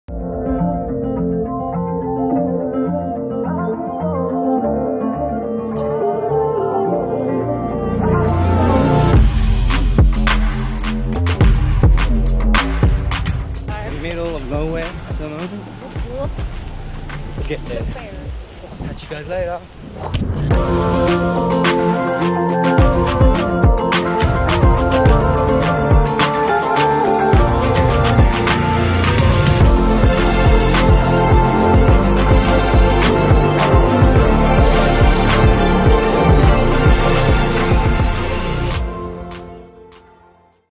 Mt.Bishop summit
Another adventure day with another beautiful scenery & peaceful by nature so we decided to head back to Wilsons Prom to hike the 2nd summit at Mt.Bishop which is only a 3.7km hike lets get it!